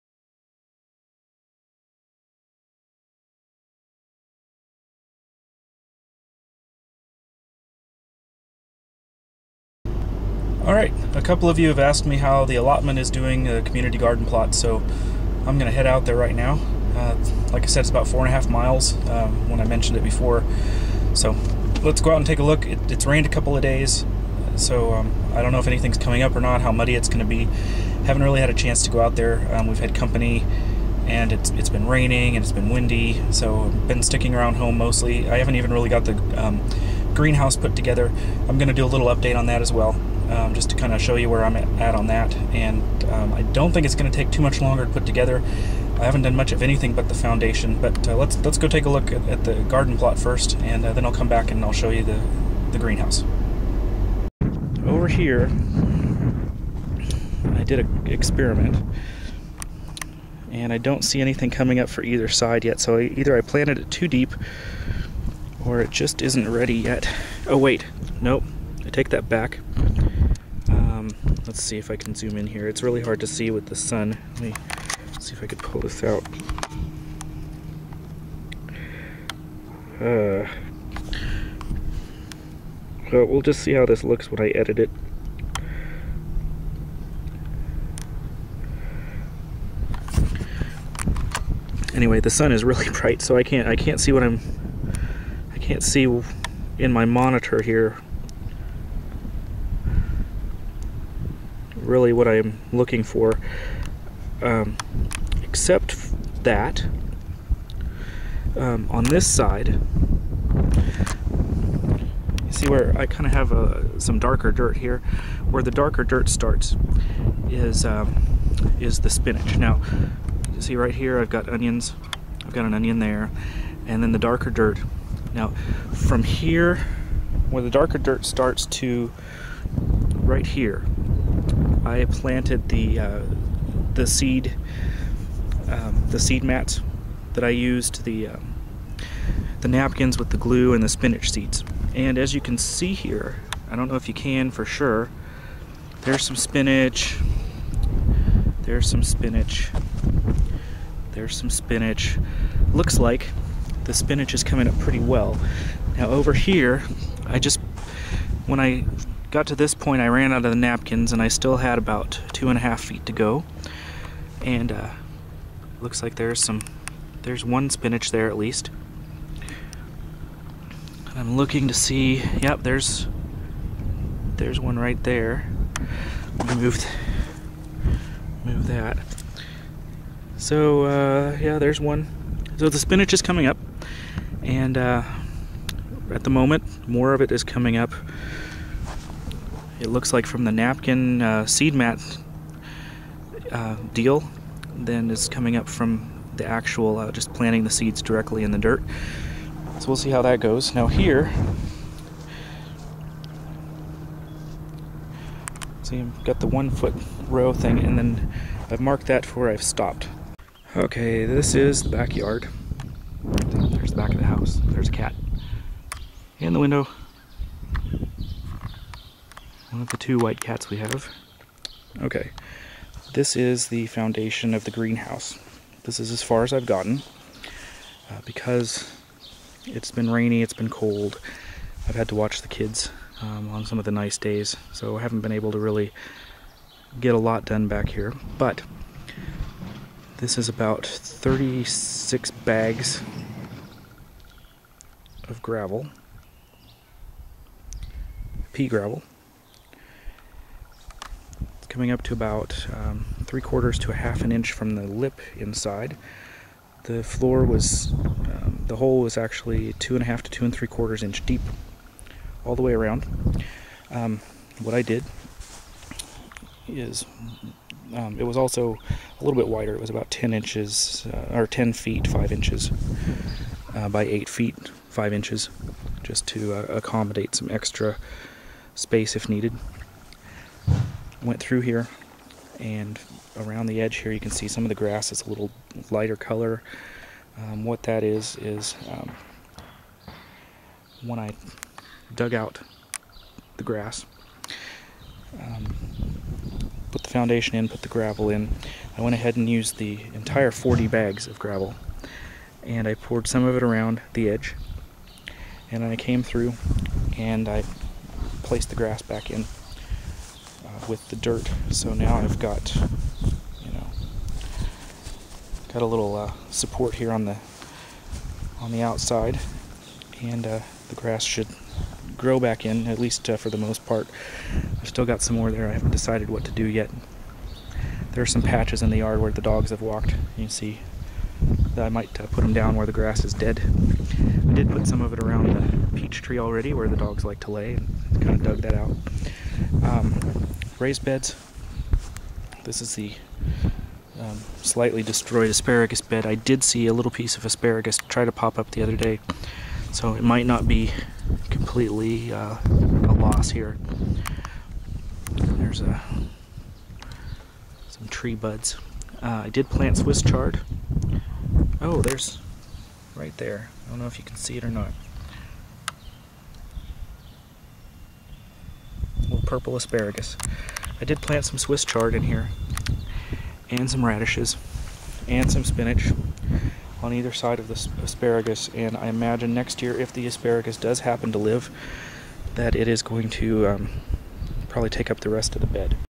2010 Garden Vlog #8
I had plenty more footage, but the wind just messed up the audio too much for it to mean anything. The video shows the spinach, which I planted half on the seed mats I made and half directly into the soil. It looks like the seed mats and direct sown are both doing well. I also was going to show that I'm 1/3 of the way through the community plot. I've planted oak leaf lettuce, broccoli, cauliflower, swiss chard, carrots, radishes - all to go along with the onions and spinach from before. I still need more radishes, turnips, beets, kholrabi, then I should be half way through. I bought some corn (kandy korn) variety that I'll plant a few rows of when it gets warmer, and in the rest of the space will be tomatoes and peppers. There will be tomatoes and peppers at home, too, as well as cucumbers, watermelon, pumpkins, and tigger melons. 3 more weeks until last frost so it's almost time to start moving the seedlings out.